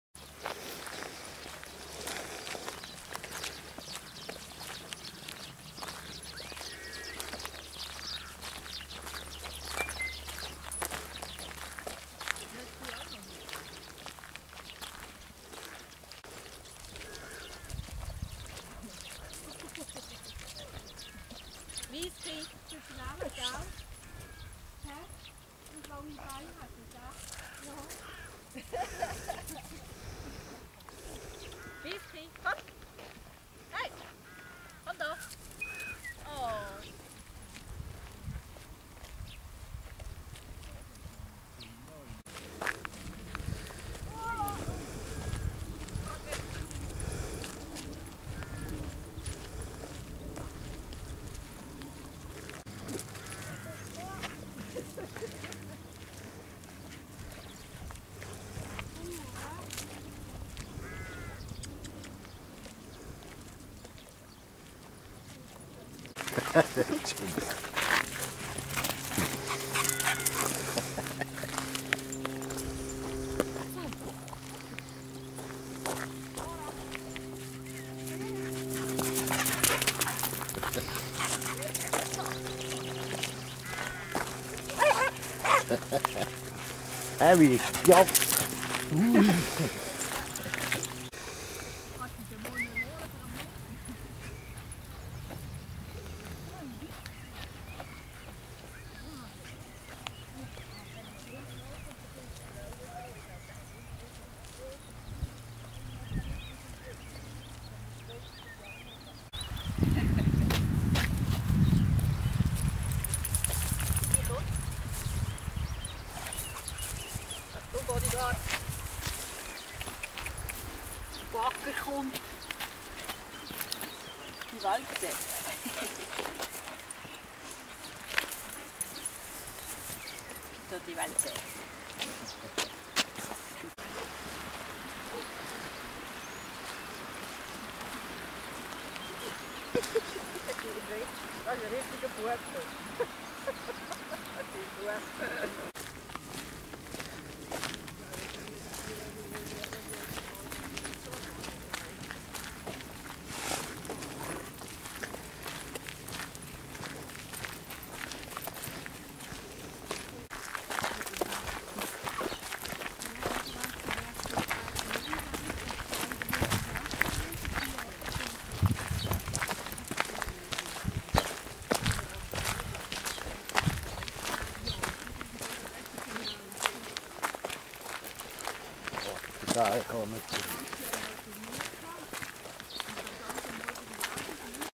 Podencos and Galgos
Our dogs love to walk together.